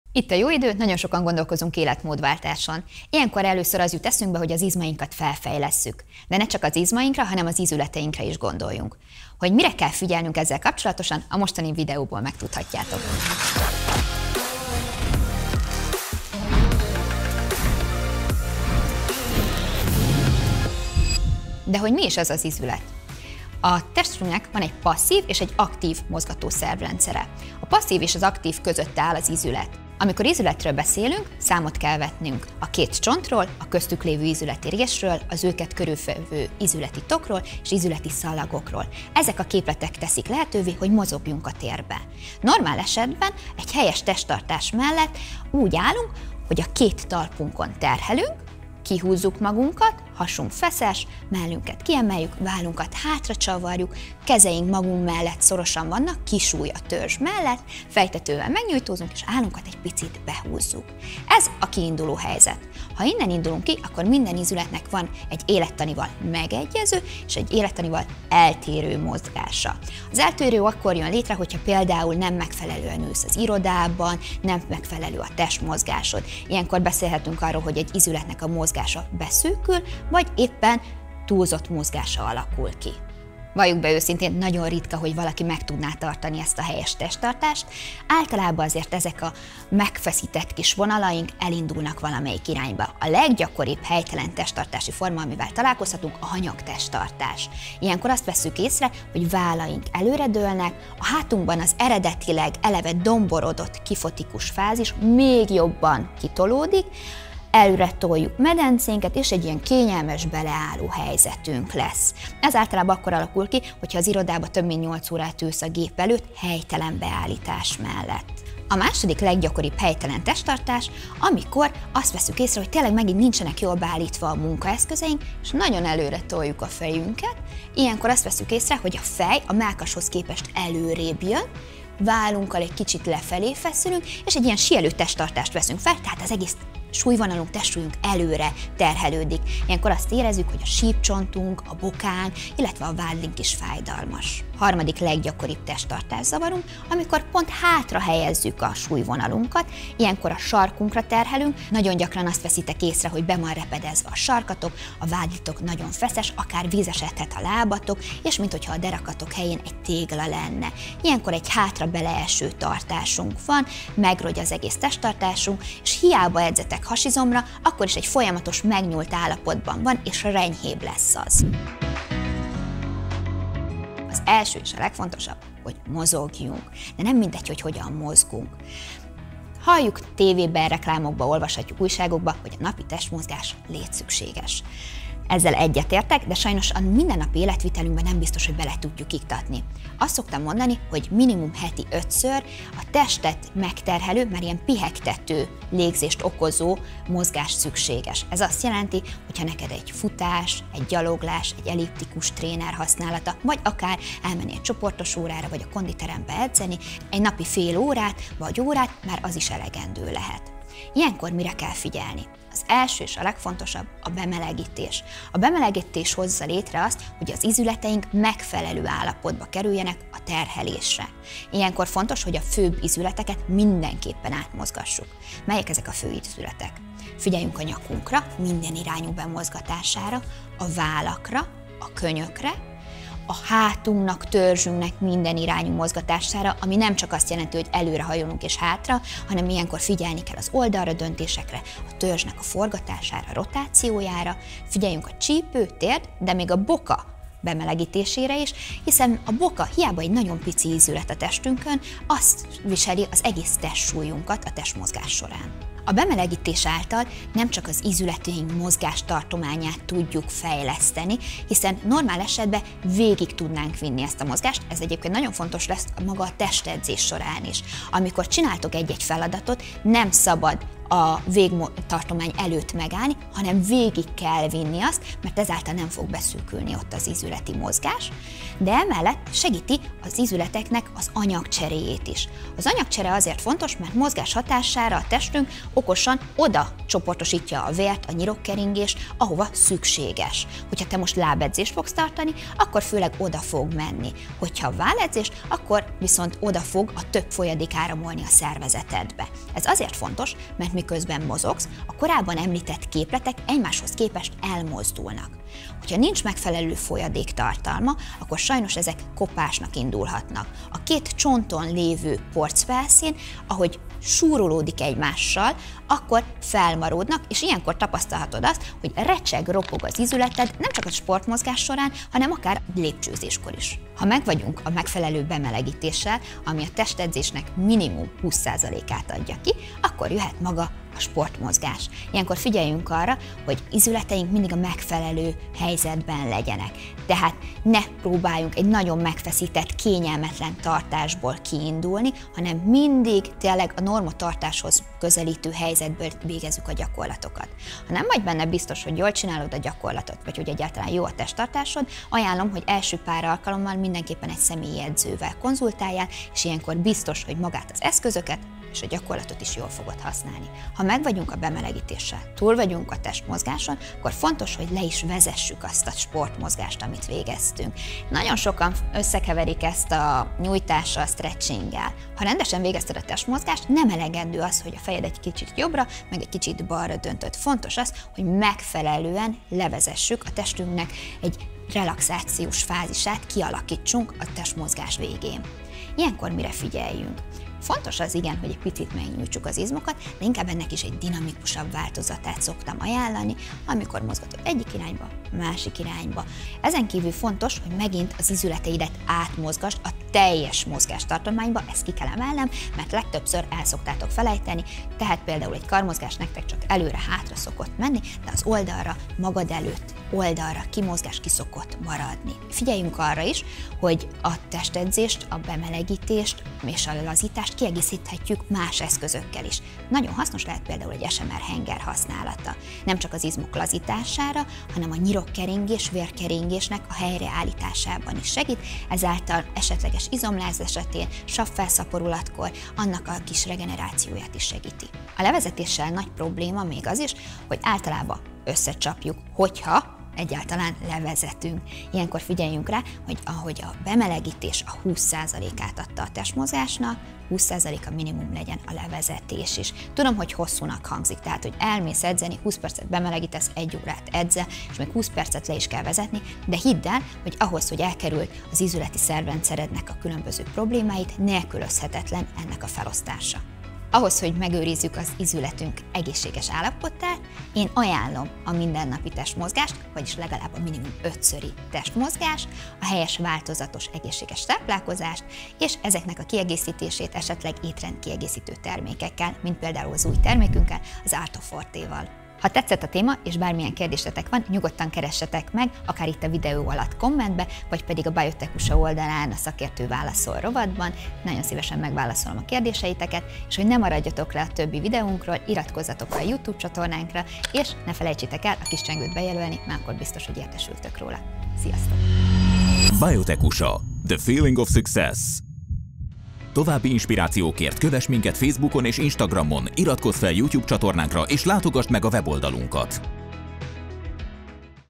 Húzd ki magad! 🧓 - íme a tökéletes testtartás, bemelegítés és nyújtás titka | BioTechUSA
A mai rohanó világban, amikor az asztal előtt görnyedünk, állandóan a mobilunkat nyomkodjuk, mozgásszegény életmódot élünk beszélhetünk egyáltalán a helyes testtartásról? 🧓👵

A helytelen testtartás nem csak esztétikai gondokat hordoz, de komoly elváltozásokat is okozhat.
A következő videóban a helytelen testtartás intő jeleit, hosszútávú következményeit, megelőzési lehetőségeit, valamint a mozgáshoz elengedhetetlen bemelegítés és nyújtás fontosságát mutatjuk be! 🤸‍♀️

További hasznos tanácsokért keresd Pajk Melitta szakértőnket a Szakértő válaszol felületen

Egészítsd ki ízületi porcaid védelmét az Arthro Forte termékcsaláddal!